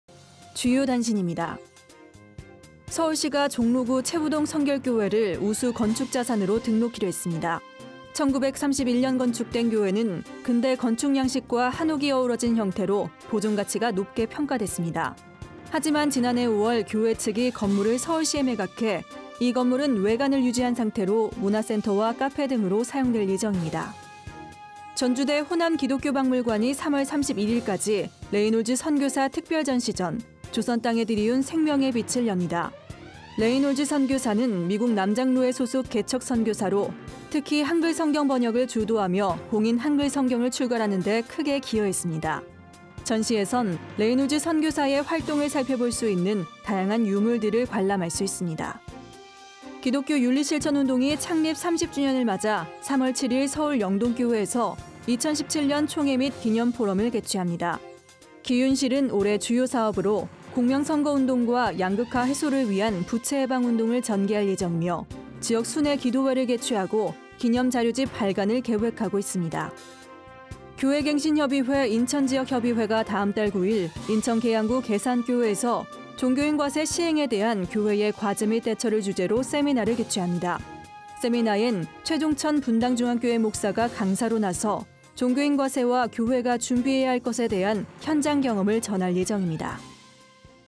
[GOODTV NEWS 20170227] 주요 단신
주요 단신입니다.

1. 서울시가 종로구 체부동성결교회를 '우수건축자산'으로 등록키로 했습니다. 1931년 건축된 교회는 근대 건축양식과 한옥이 어우러진 형태로 보존가치가 높게 평가됐습니다. 하지만 지난해 5월 교회 측이 건물을 서울시에 매각해 이 건물은 외관은 유지한 상태로 문화센터와 카페 등으로 사용될 예정입니다.

2. 전주대 호남기독교박물관이 3월 31일까지 레이놀즈 선교사 특별전시전 '조선 땅에 드리운 생명의 빛'을 엽니다. 레이놀즈 선교사는 미국 남장로회 소속 개척 선교사로, 특히 한글성경번역을 주도하며 공인 한글성경을 출간하는데 크게 기여했습니다. 전시에선 레이놀즈 선교사의 활동을 살펴볼 수 있는 다양한 유물들을 관람할 수 있습니다.

3. 기독교윤리실천운동이 창립 30주년을 맞아 3월 7일 서울영동교회에서 '2017년 총회 및 기념포럼'을 개최합니다. 기윤실은 올해 주요사업으로 공명선거운동과 양극화 해소를 위한 부채해방운동을 전개할 예정이며, 지역순회 기도회를 개최하고 기념자료집 발간을 계획하고 있습니다.

4. 교회갱신협의회 인천지역협의회가 다음 달 9일 인천 계양구 계산교회에서 '종교인 과세 시행에 대한 교회의 과제 및 대처'를 주제로 세미나를 개최합니다. 세미나엔 최종천 분당중앙교회 목사가 강사로 나서 '종교인 과세와 교회가 준비해야 할 것'에 대한 현장 경험을 전할 예정입니다.